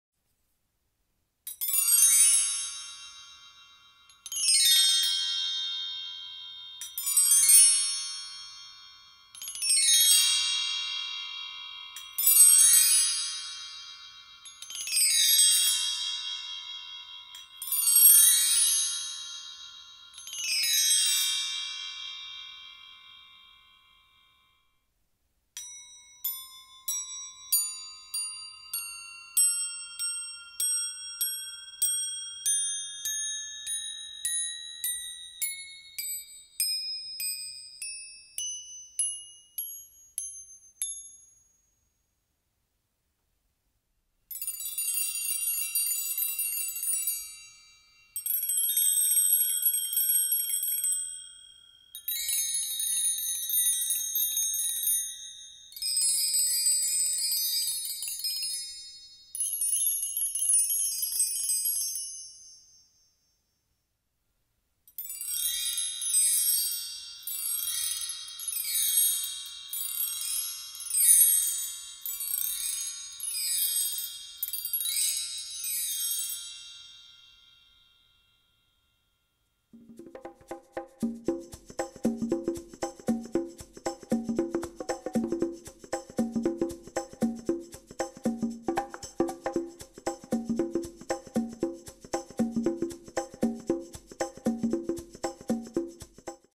【SOUND CHECK】LP450 Bell Tree / LP【BELLTREE】
パーカッションのサウンドチェック。LPのベルツリーです。ブラスのベルが26連になっています。

Recoder : TASCOM DR-40